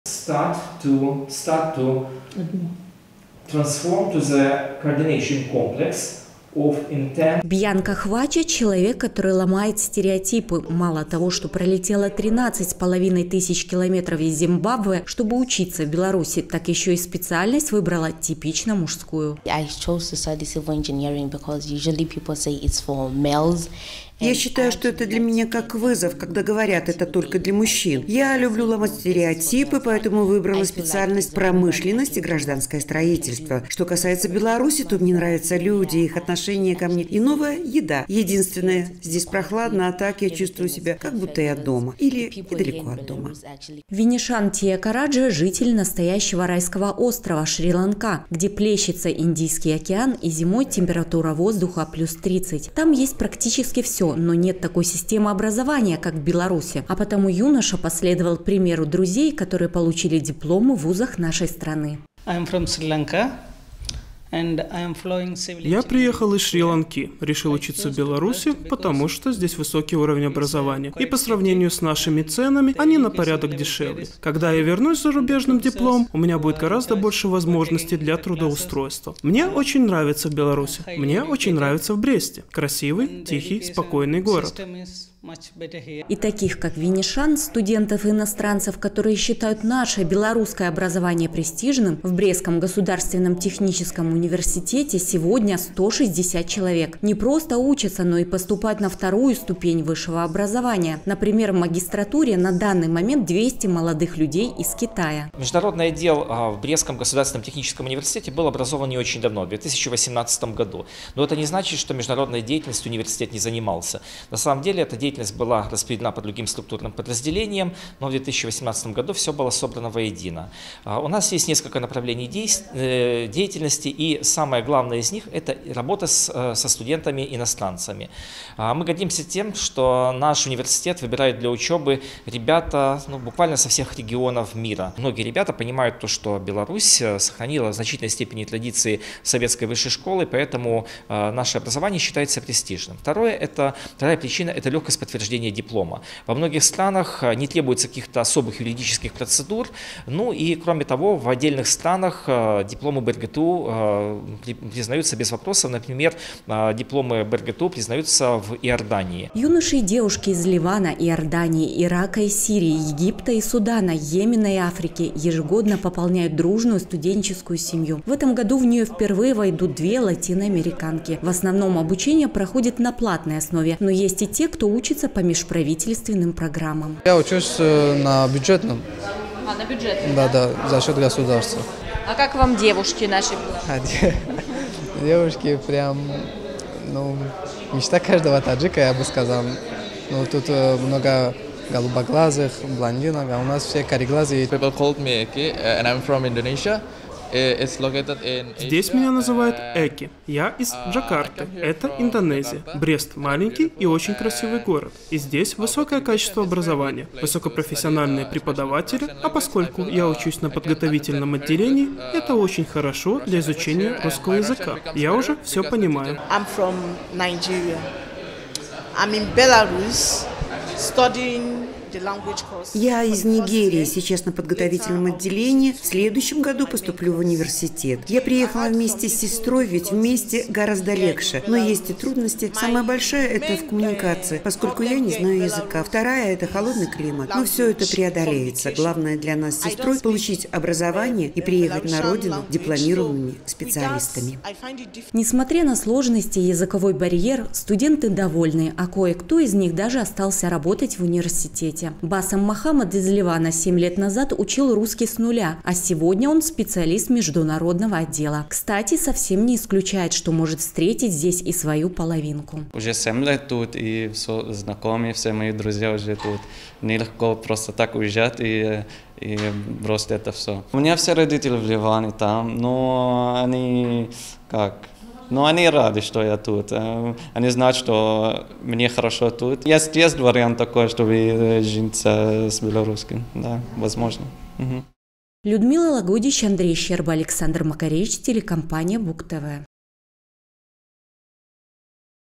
2021-11-25 г. Брест. Жизнь, учеба и работа иностранных студентов в БрГТУ. Новости на Буг-ТВ.
Пока бушуют страсти на границе с Польшей, а тысячи беженцев находятся в положении вынужденного ожидания и неопределенности – в Брестском Государственном Техническом Университете студентов – иностранцев встречают как родных. Здесь  они  вливаются в международную университетскую семью, обретают второй дом и близких друзей, ну и самое главное - получают высшее образование, которое ценится и считается престижным во многих уголках мира. Некоторые из них даже остаются работать.